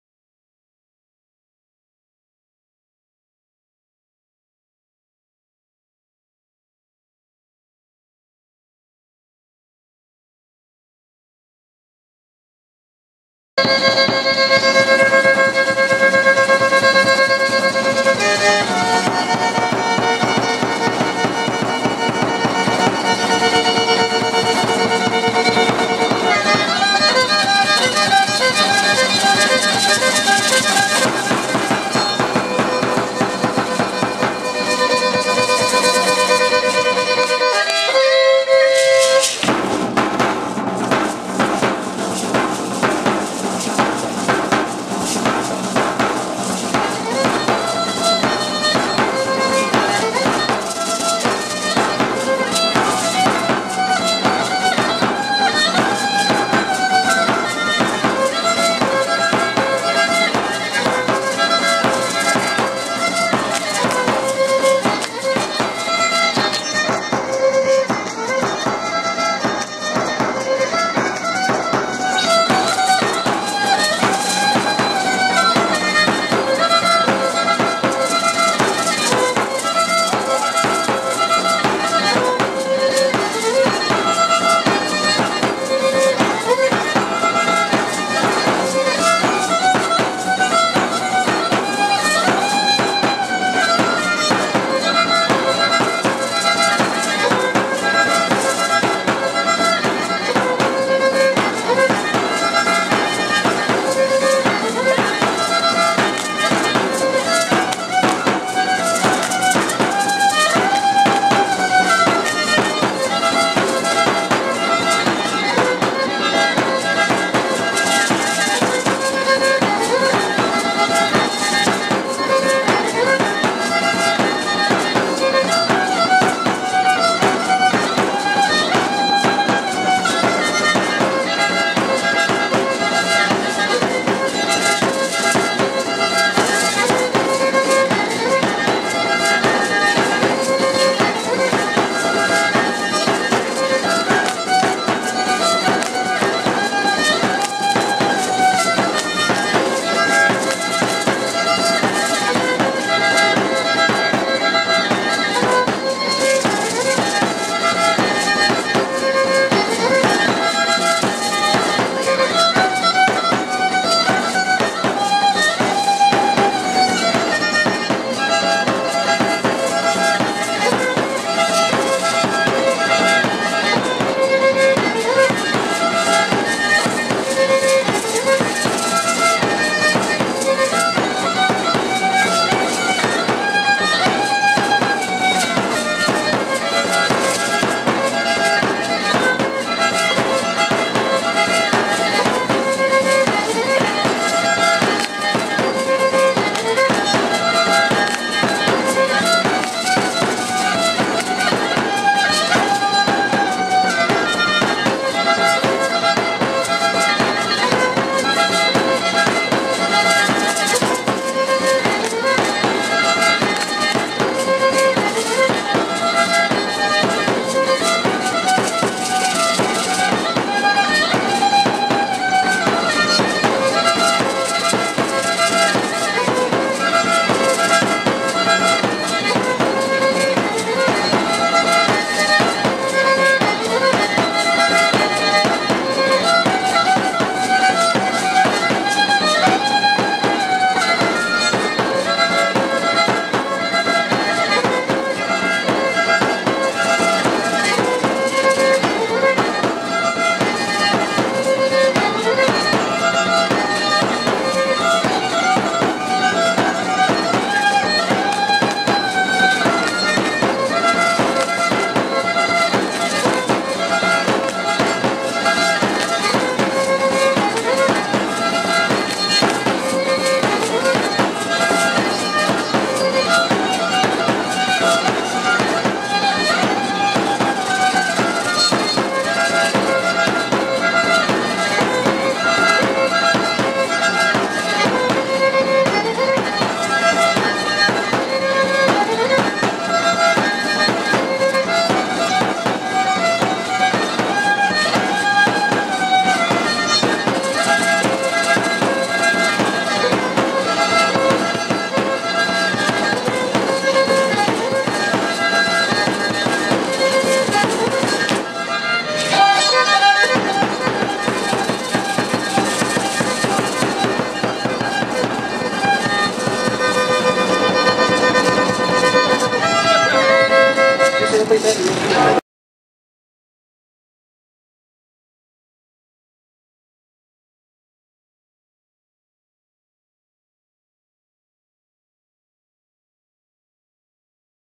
X Encuentro de Danza de La Laguna. Danza de La Col Solidaridad de Cirilo.
X Encuentro de Danza de La Laguna. En la Cd. Gómez Palacio, Dgo. el 21 de diciembre de 2014.